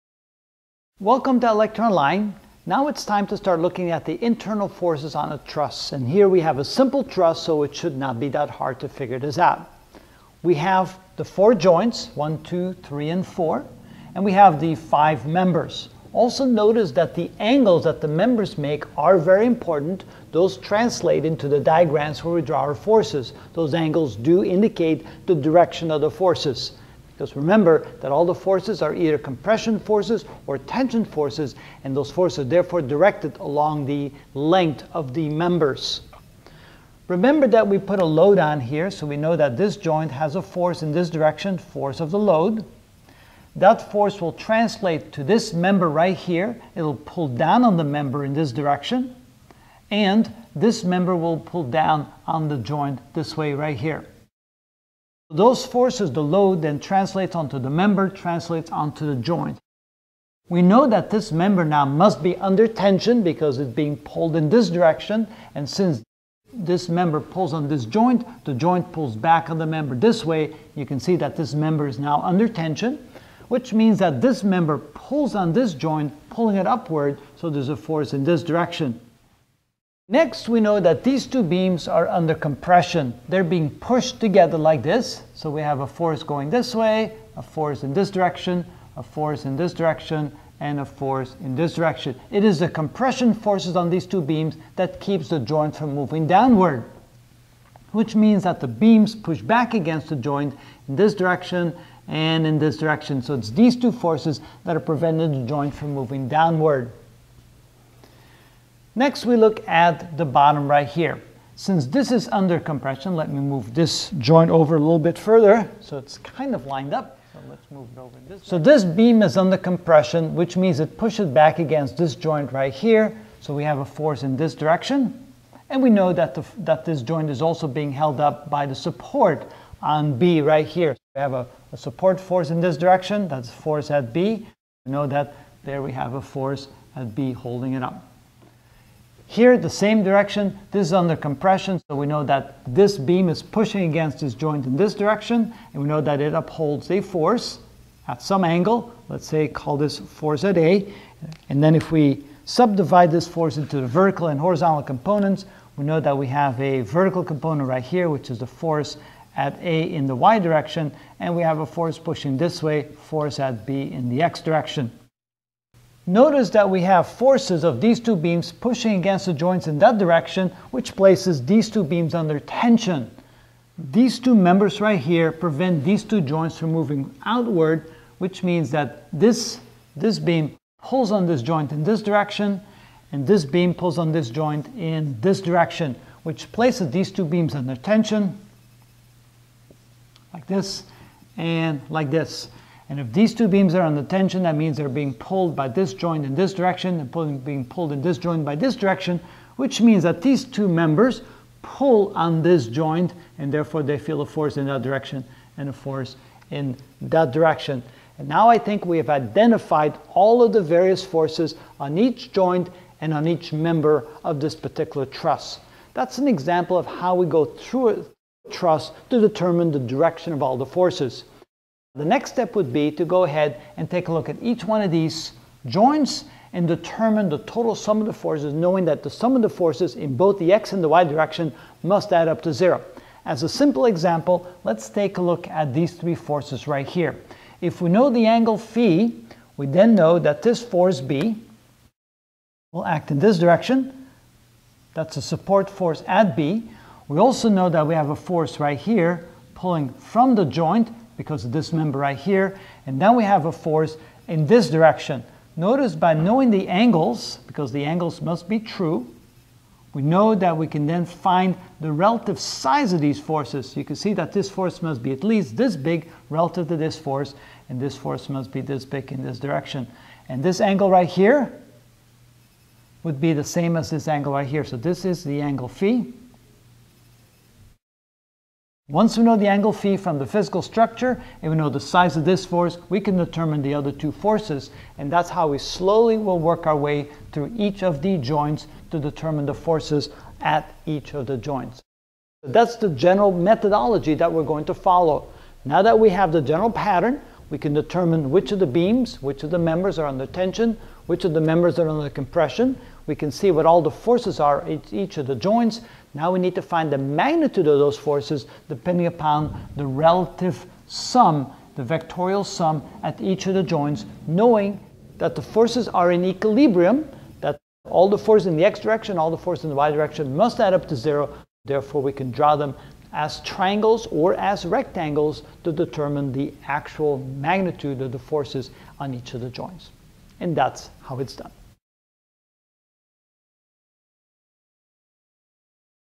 Mechanical Engineering: Trusses, Bridges & Other Structures (9 of 34) What are Internal Forces?
In this video I will explain the internal forces of tension and compression on a truss.